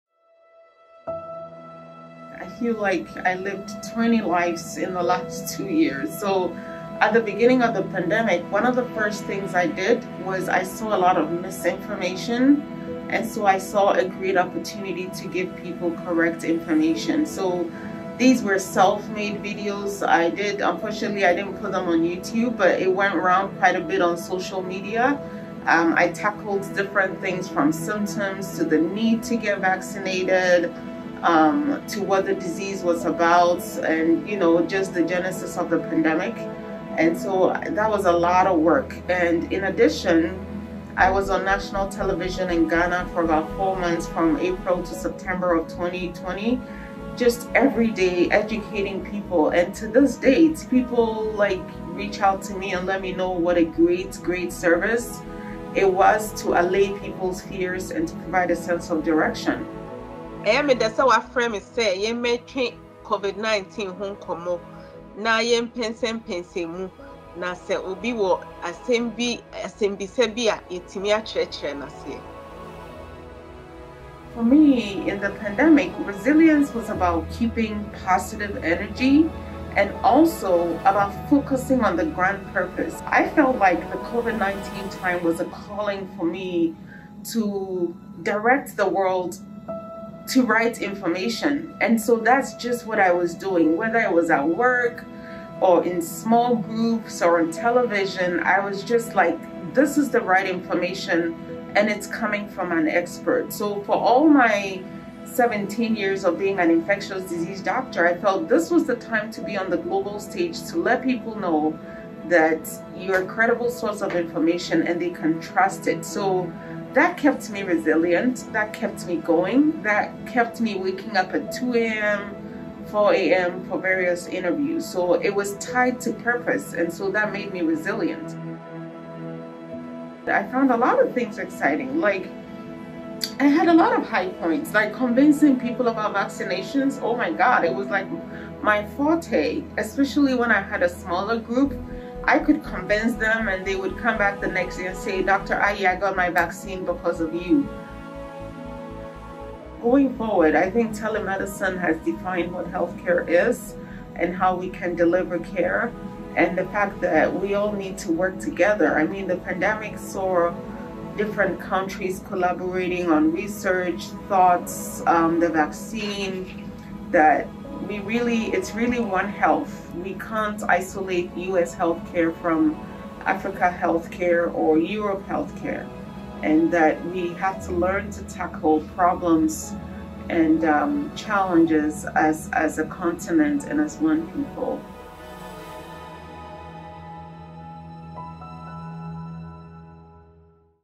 Dr. Bertha Serwa Ayi: Employee Spotlight Essentia Health, Fargo, ND, USA
Dr. Bertha Ayi has always dedicated her time and life to sharing her wealth of medical knowledge with the world in print, video and in lectures. In this video Miss Kelsey Mix, Media Relations Specialist at Essentia Health West explores the various ways Dr. Bertha Serwa Ayi made global impact at the height of the COVID-19 pandemic. The values of Essentia include Quality, Hospitality, Respect, Joy, Justice, Stewardship, and Teamwork. Miss Mix This was video was shared as an employee spotlight in our weekly Newsletter “The Dose”. Footage was recorded by Kelsey, edited for internal and external use by Korina Borash. I am grateful to both. I am grateful to be part of the family at Essentia Health.